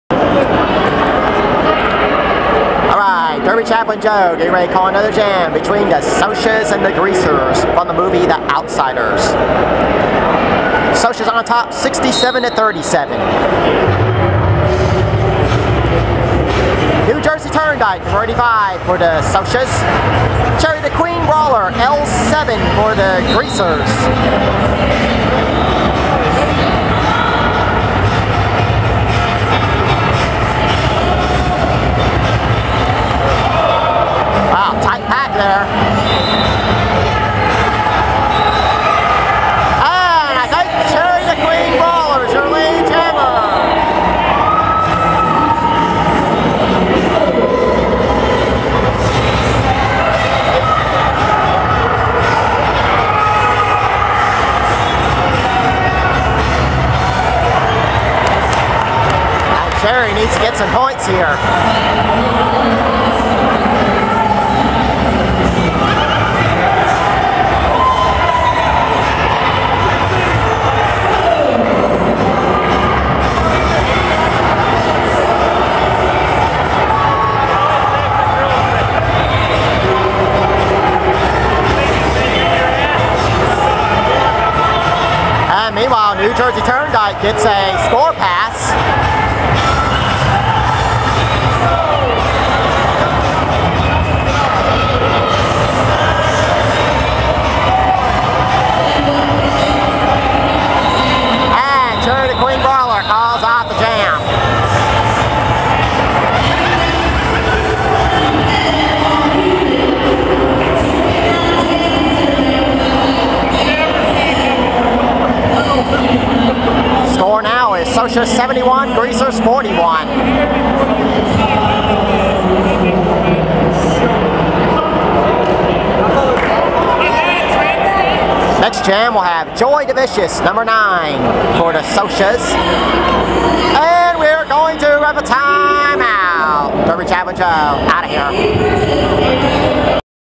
MRD Mash-Up-1/28/12-Video 18
Memphis Roller Derby-January 28, 2012

Mash-Up Bout:  Socs vs. Greasers

Jammers:  New Jersey Turndyke (Socs) and Cherry The Queen Brawler (Greasers)